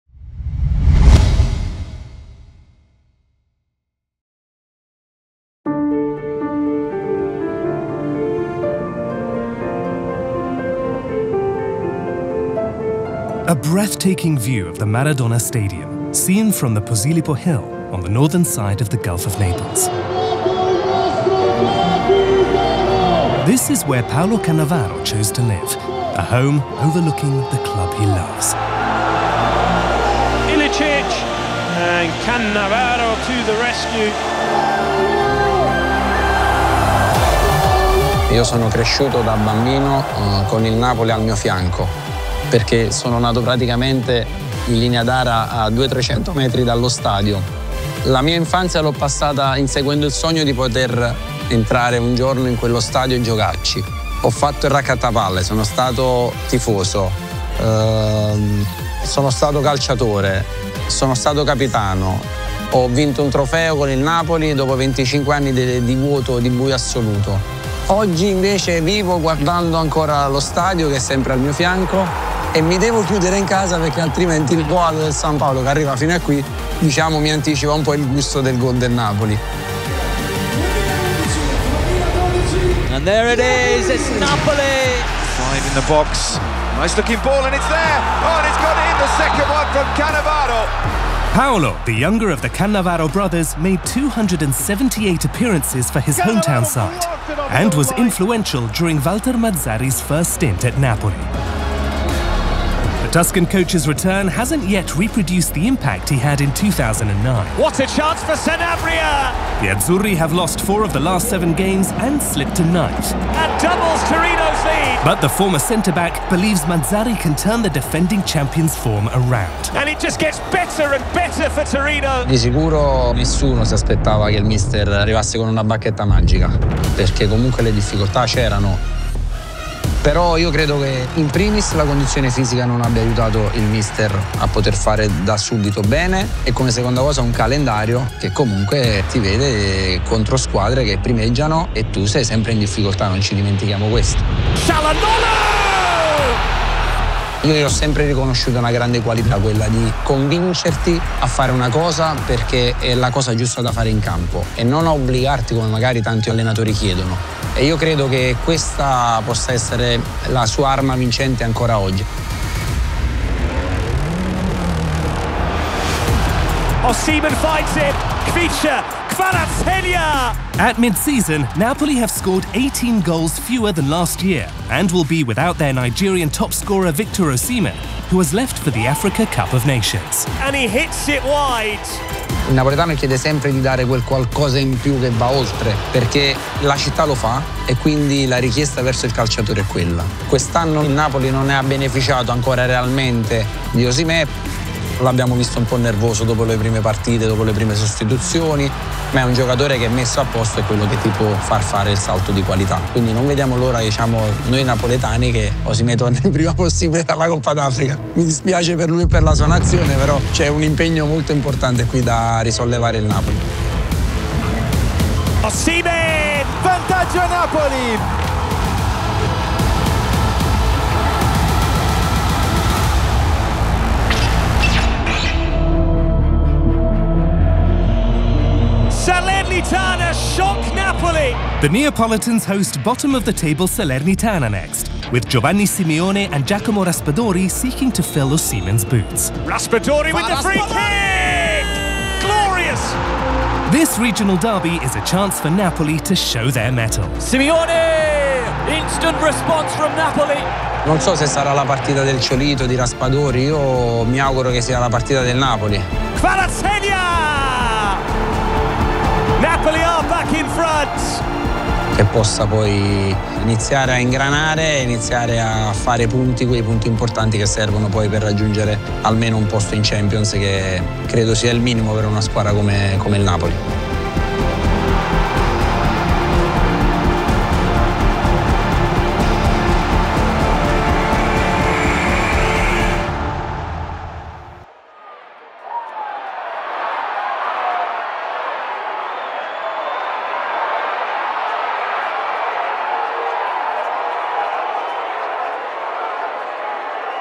Napoli Legend on the Champions’ Woes This Season | A Chat with Cannavaro | Serie A 2023/24
278 matches with Napoli and two trophies won after one of the club’s darkest periods, Paolo Cannavaro has made a name for himself in his hometown. The youngest of the Cannavaro siblings, Paolo stepped out of his brother’s shadow, leaving a permanent mark in the fans’ hearts even after hanging up his boots | Serie A 2023/24